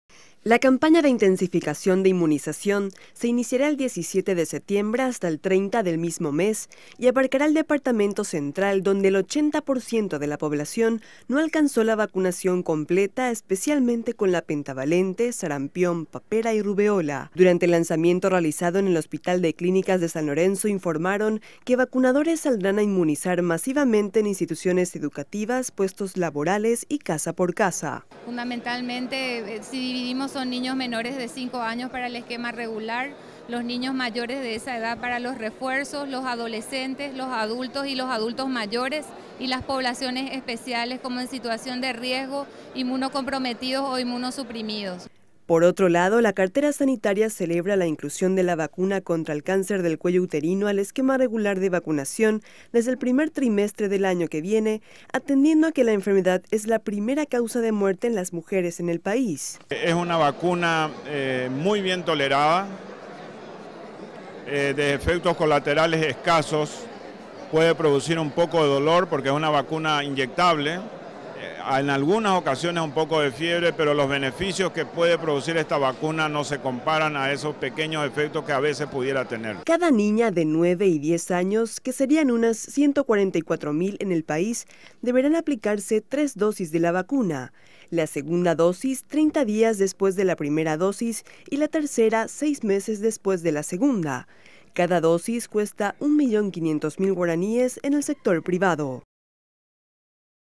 VACUNACION
EL MINISTRO DE SALUD EXPLICA QUE DE NADA SIRVE CONTAR CON LA MAYOR CANTIDAD DE VACUNAS PARA EVITAR ENFERMEDADES GRAVES PERO PREVENIBLES SI NO SE ALCANZAN LOS INDICES IDEALES DE VACUNACION COMO SUCEDE ACTUALMENTE EN EL PAIS. PARA AUMENTAR LA COBERTURA LANZARON LA CAMPAÑA DE INTENSIFICACION DE INMUNIZACION PARA EL DEPARTAMENTO CENTRAL.